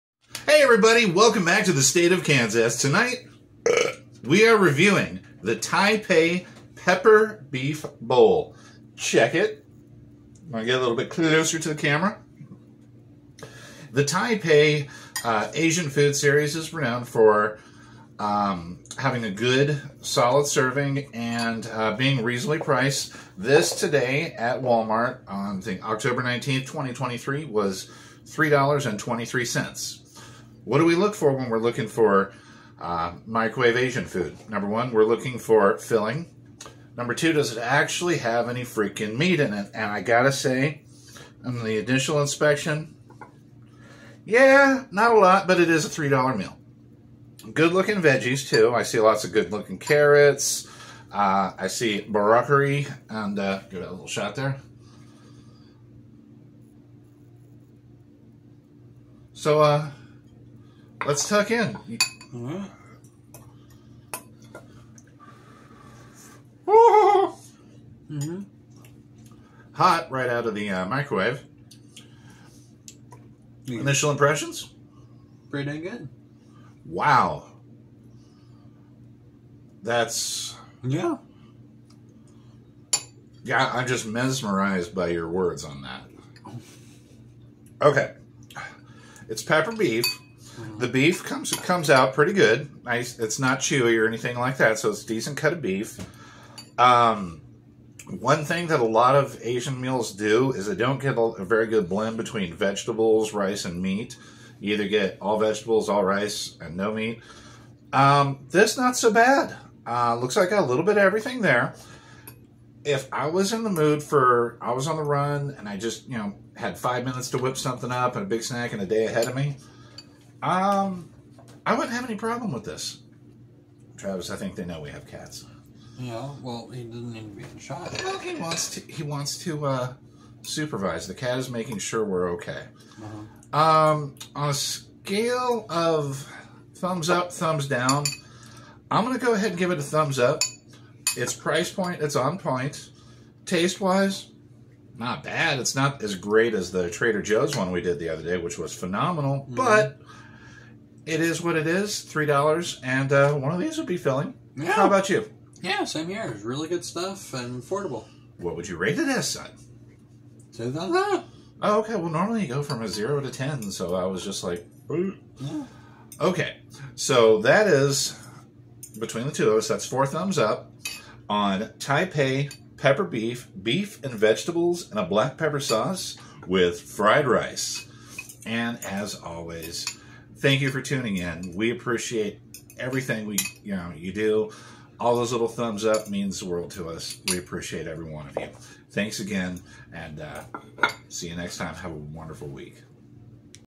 Tai Pei Pepper Beef
join your favorite to reviewers as we tackle the most difficult subjects of all time. who has the best microwave Chinese food?